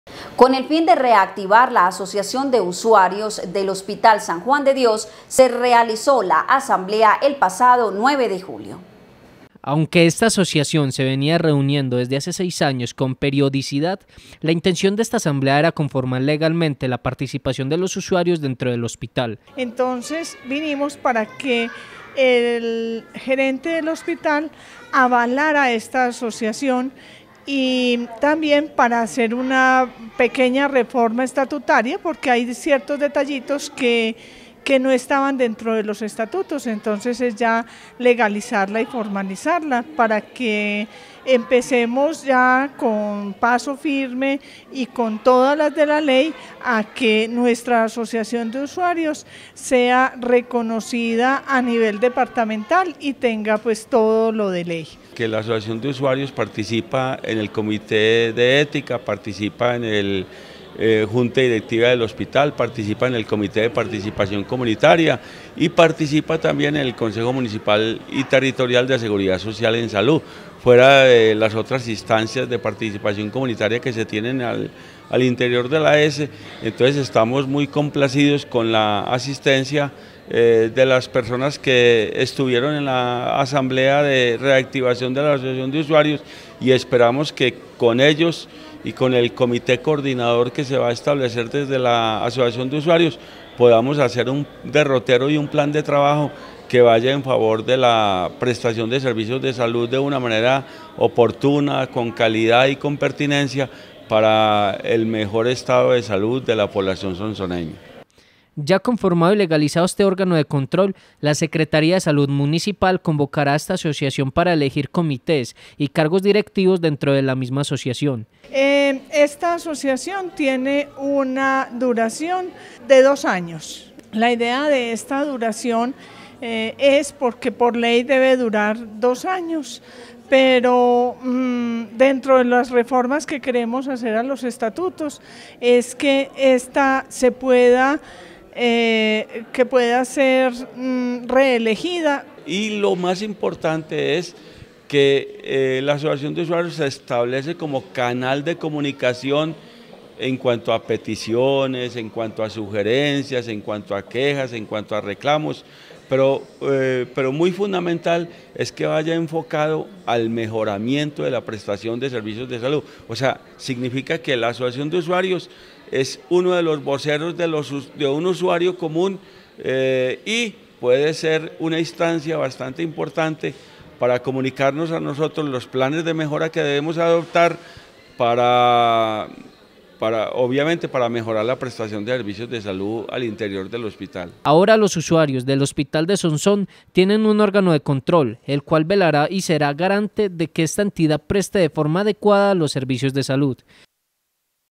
REACTIVACIÓN ASOCIACIÓN DE USUARIOS HOSPITAL
Con el fin de reactivar la asociación de usuarios del hospital de Sonsón se realizó la asamblea el pasado 9 de julio.